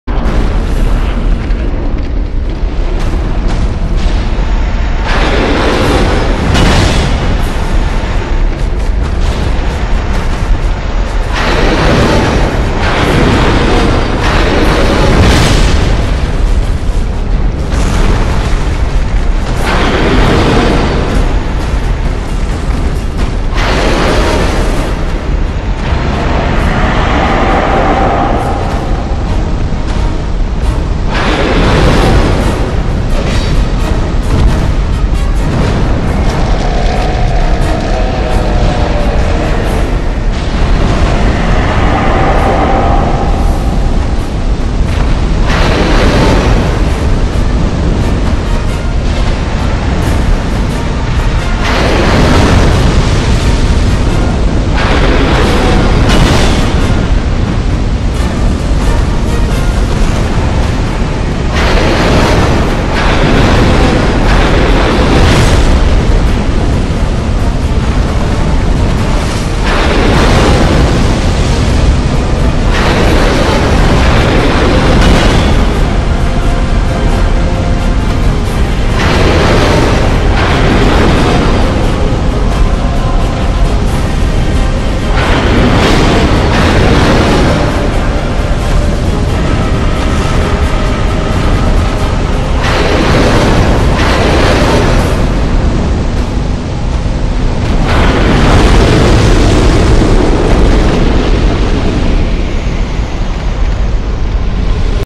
Poison works on suits of armor
It makes them jiggle too